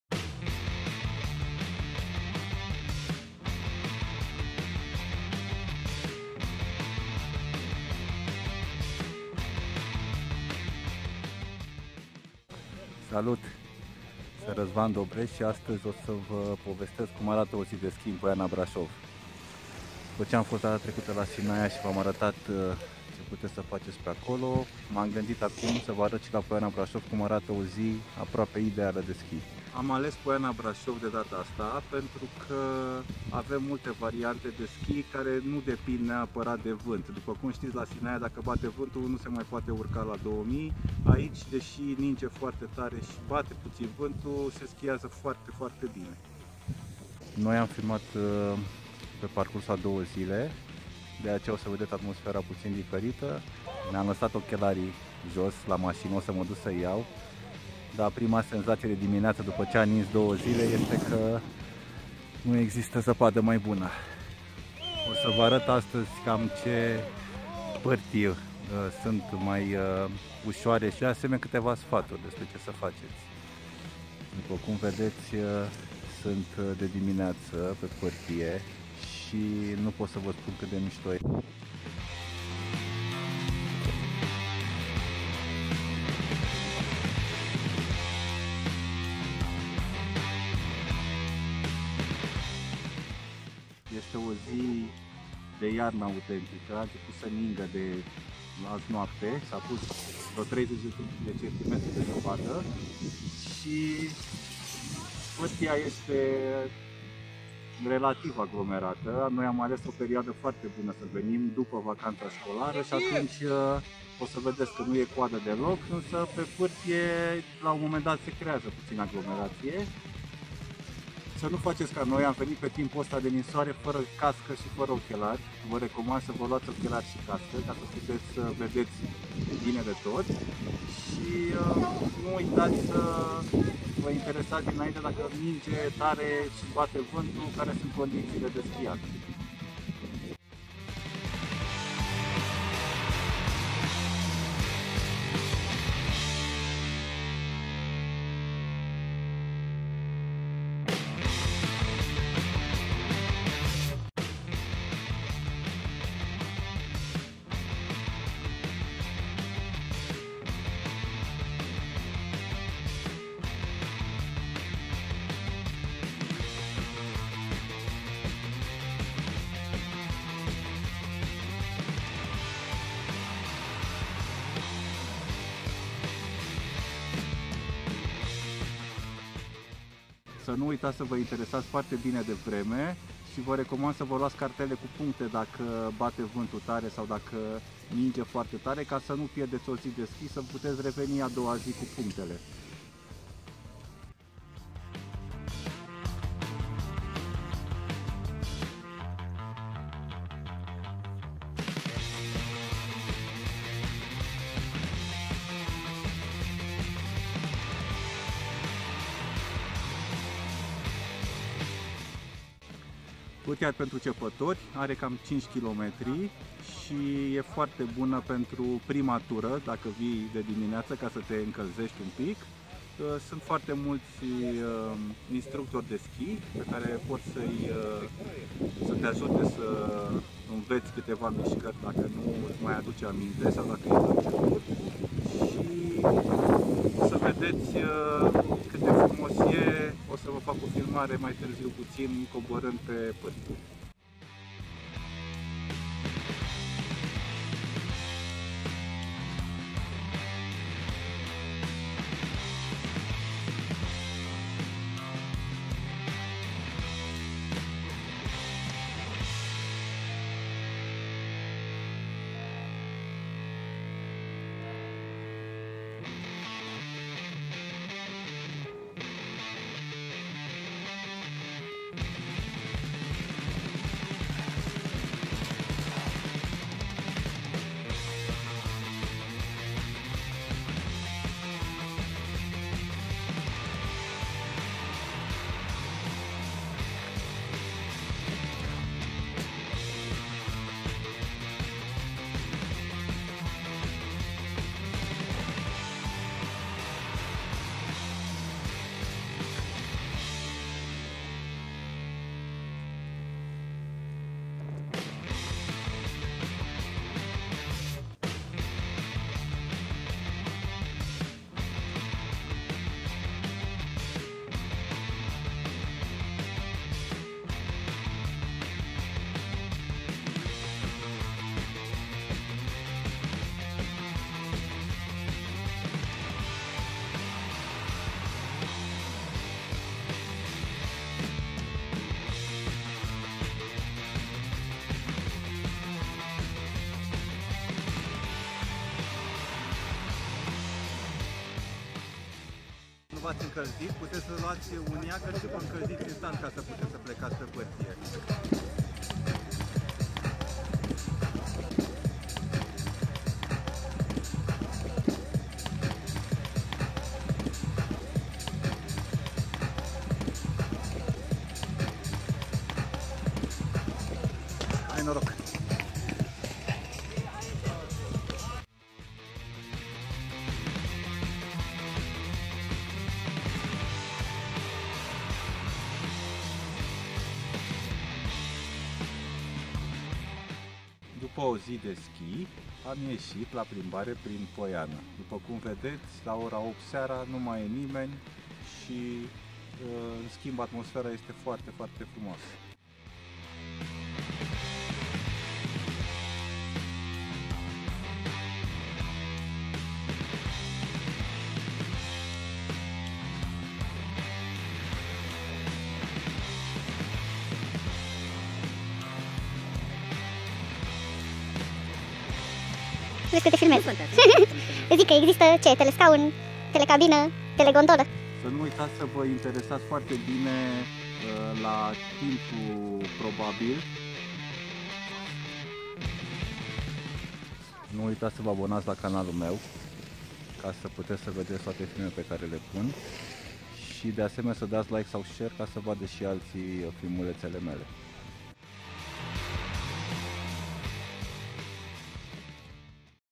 Ski la Poiana Brașov - Februarie 2019
O zi de ski la Poiana Brașov în februarie 2019. Detalii despre pârtii, câteva sfaturi și atmosfera din stațiune.

Eu am fost în timpul săptămânii, după vacanța școlară și a fost perfect. Nu a fost în nicio zi coadă la telegondolă, pârtia nu era aglomerată,. Am reușit să găsim mereu loc de parcare aproape de pârtie pentru a nu căra schiurile foarte mult.

Scrieți-mi în comentarii care a fost cea mai bună zi de schi a voastră.

Nu uitați să vă abonați și să apăsați și clopoțelul de notificări ca să vedeți toate filmele postate de mine imediat ce apar.